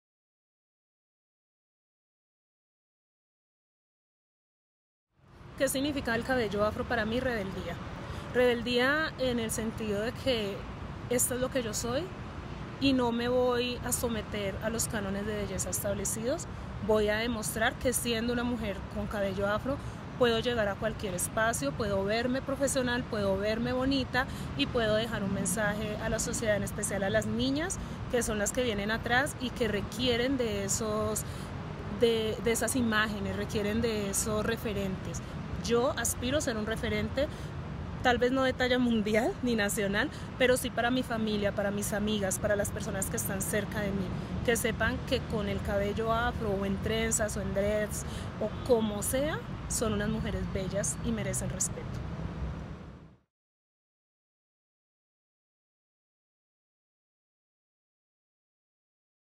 EPT: Dora Maturana Que Significa tu Cabello Para Ti
Dora le cuenta a la comunidad EPT, que significa su cabello actualmente para ella. Si quieres ver la anécdota de pelos de Dora sigue este enlace